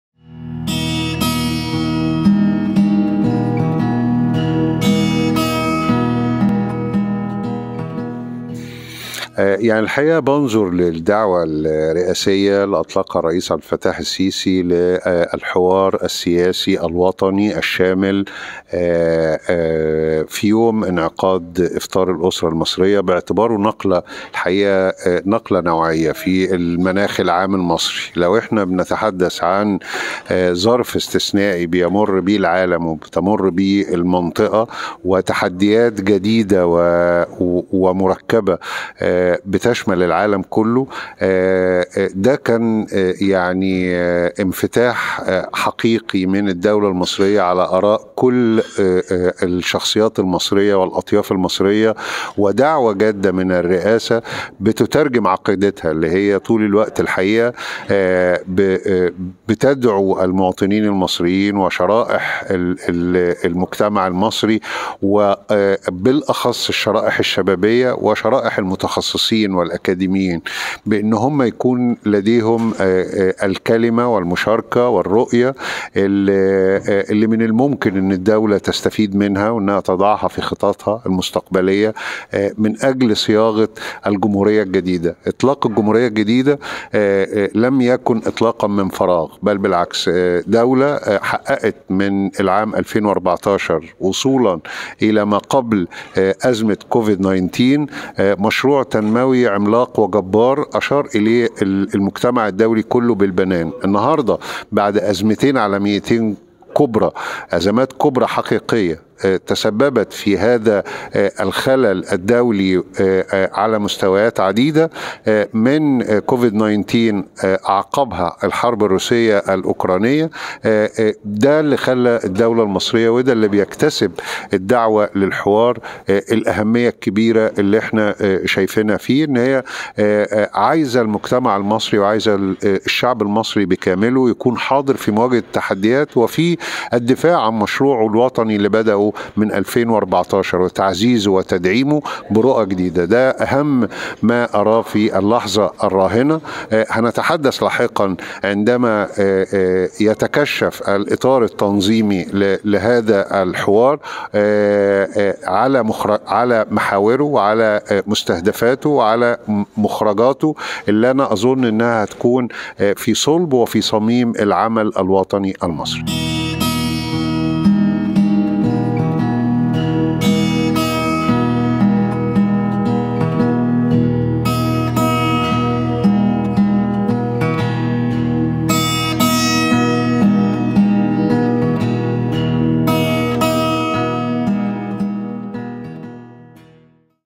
خالد عكاشة يكشف أهمية «الحوار الوطني»: نقلة نوعية لمستقبل مصر
تقرير : الوطن